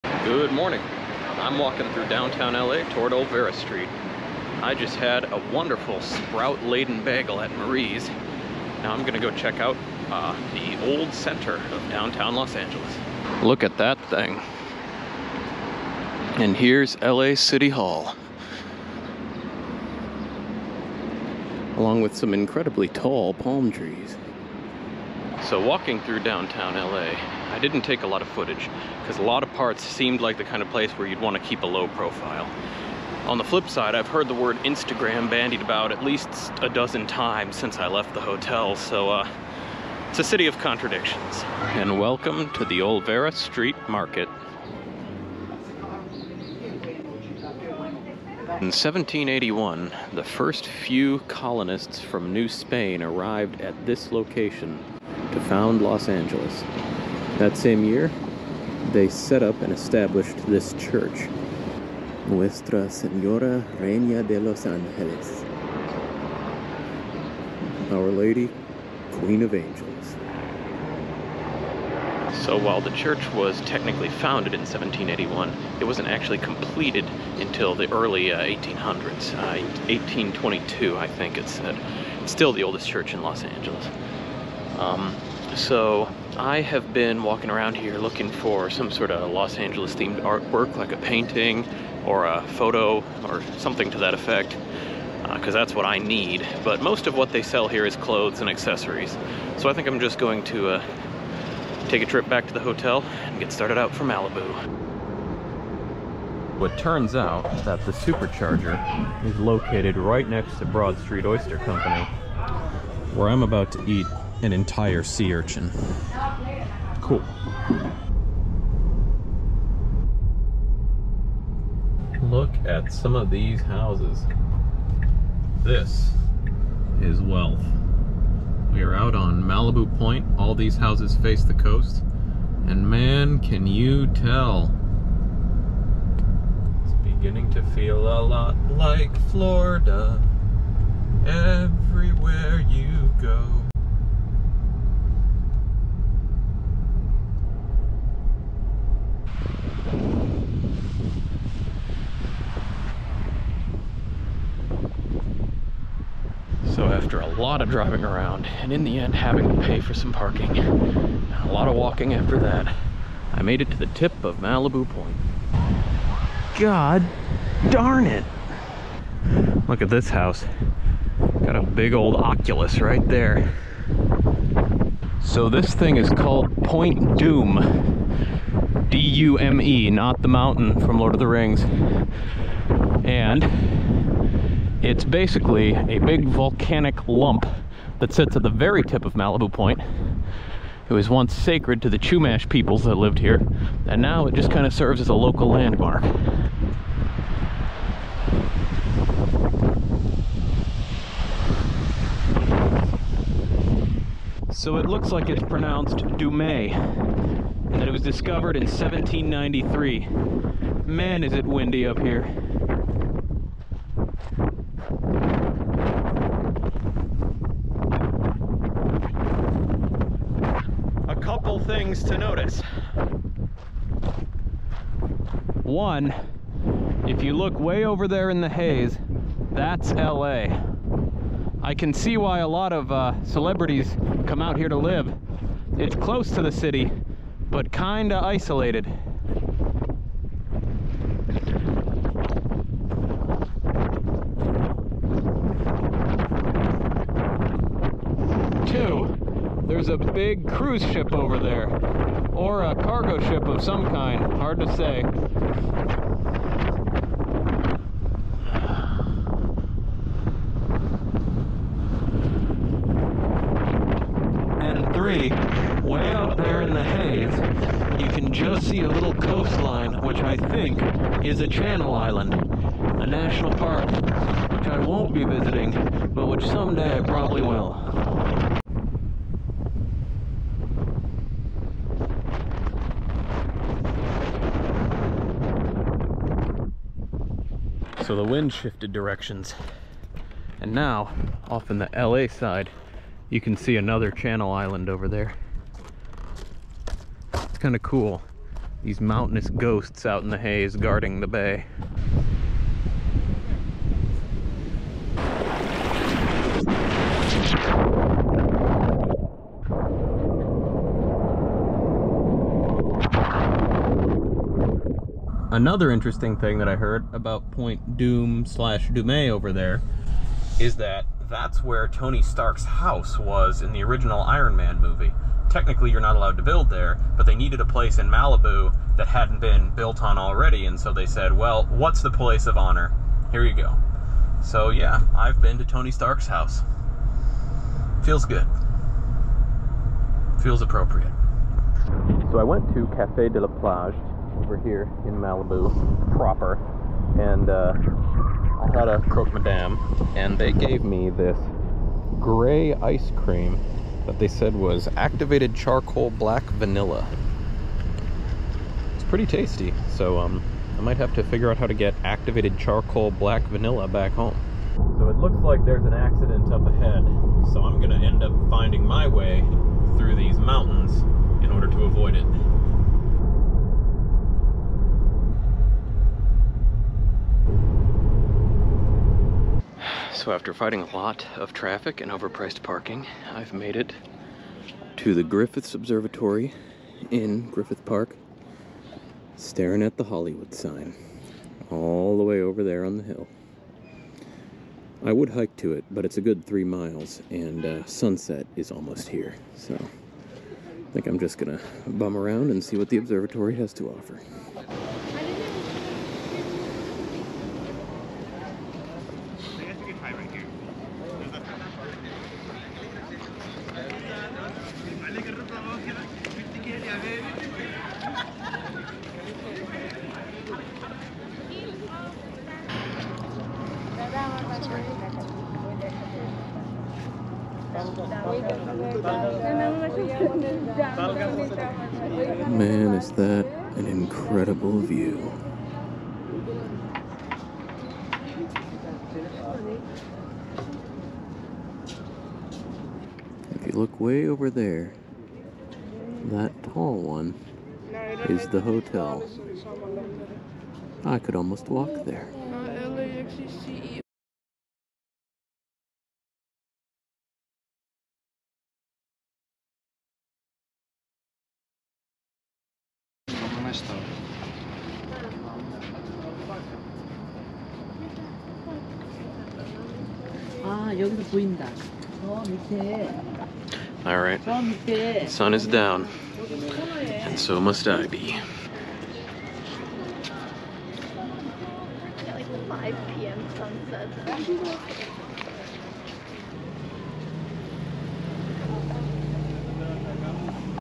Malibu and Griffith Observatory
My second day in LA, including a sea urchin, sunset time-lapse, and an accidental pilgrimage to Iron Man's house.